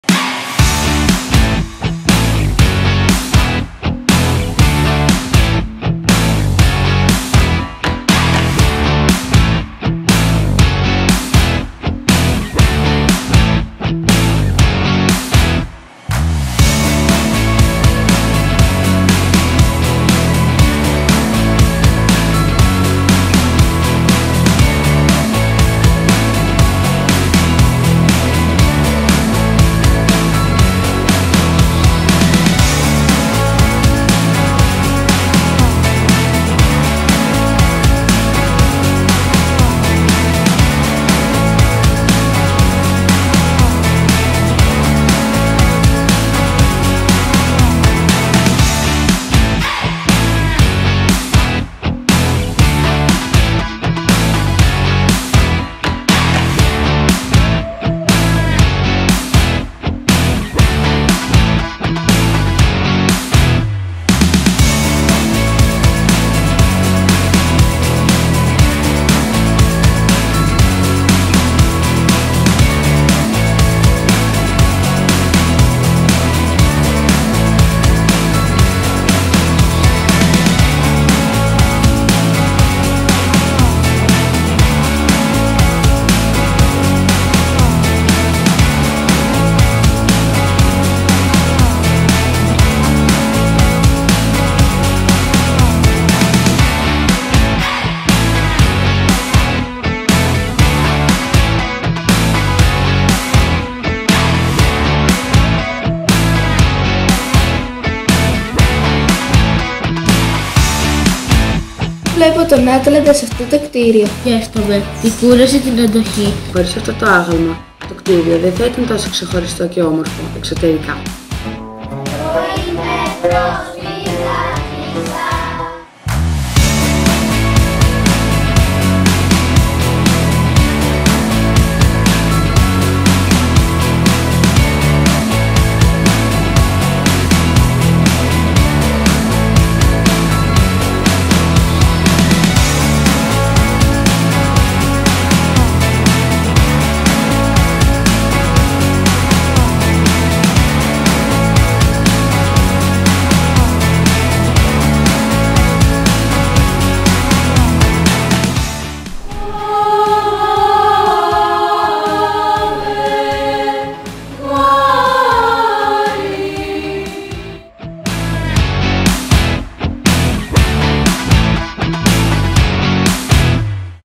Κάθε κτήριο μια ιστορία
Πάνω από 400 μαθητές και εκπαιδευτικοί από 20 σχολεία συμμετείχαν στην κοινή δράση του Cosmopolis Festival με την πρωτοβάθμια και δευτεροβάθμια εκπαίδευση Καβάλας. Επίσημη έναρξη - παρουσίαση της δράσης: 27 Ιουλίου στο 5ο γυμνάσιο Καβάλας.

Είστε όλοι προσκεκλημένοι!